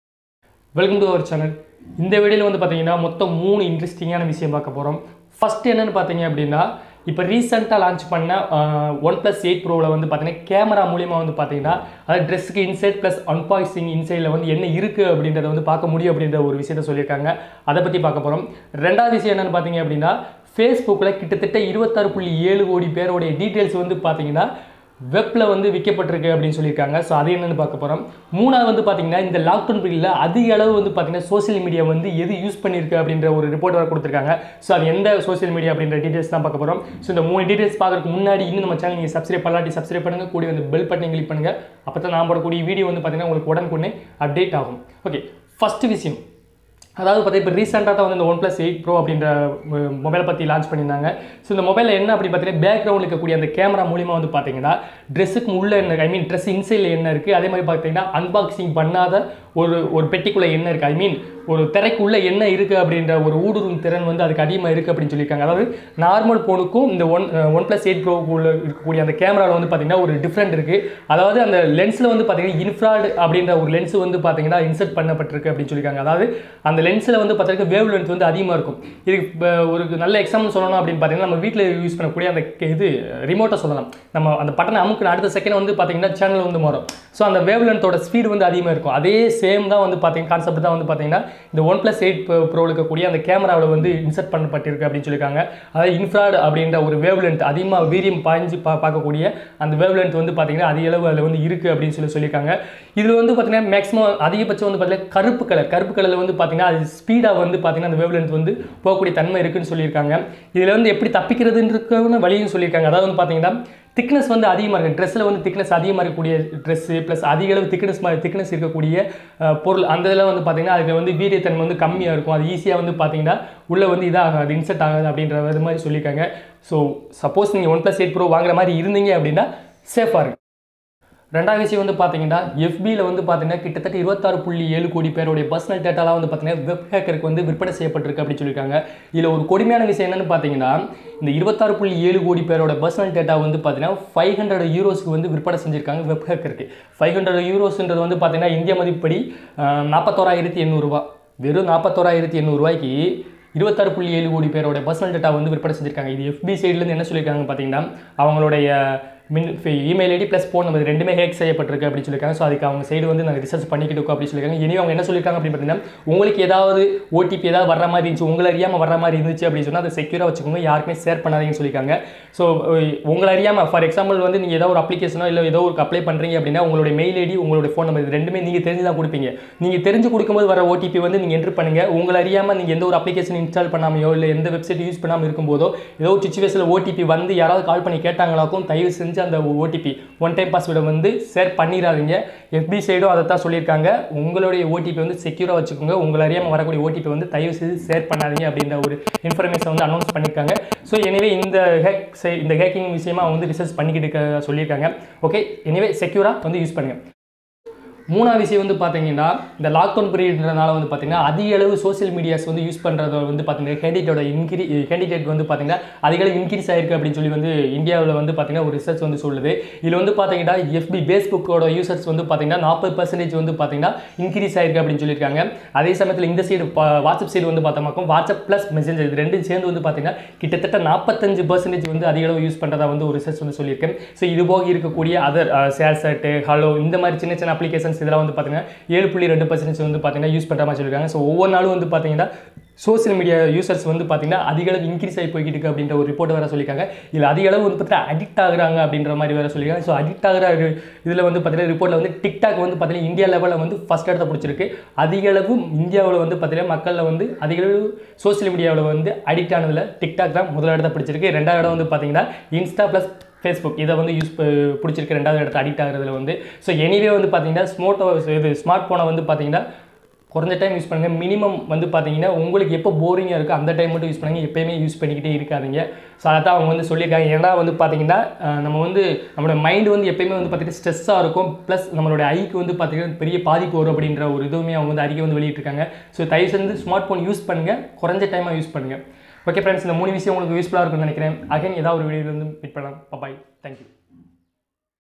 ஆடைகளை ஊடுருவி படம் எடுக்கிறதா கேமரா... உண்மை என்ன?|26.7 கோடி ஃபேஸ்புக் பயனர்களின் டேட்டா விற்பனை
Join our Telegram group now-JARS Info
This channel main focus Online course and online jobs

Government and private sectors jobs alert